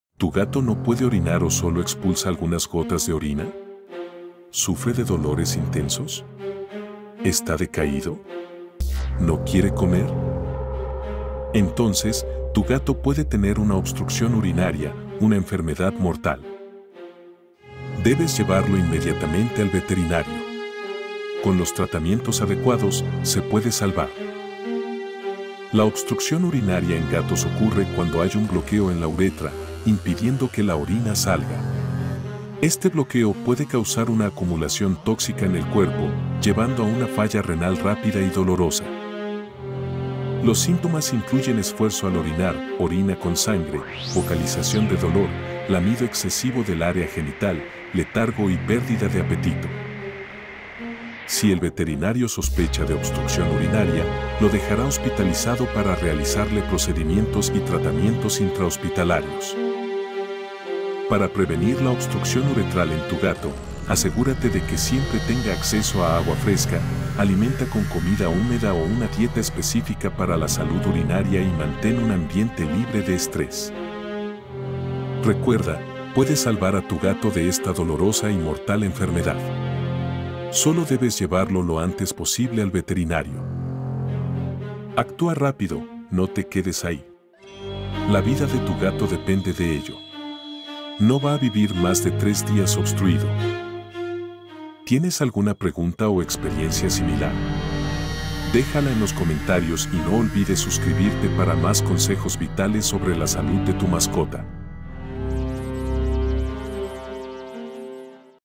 🚨 Los GATOS con OBSTRUCCIÓN URINARIA no viven más de 3 días, ¡mira rápidamente este video!
Si tu gato no puede orinar, entontes puede tener una obstrucción renal, una enfermedad mortal. En este video descubre los síntomas de esta enfermedad, lo que debes hacer si sospechas que la tiene y cómo prevenirla.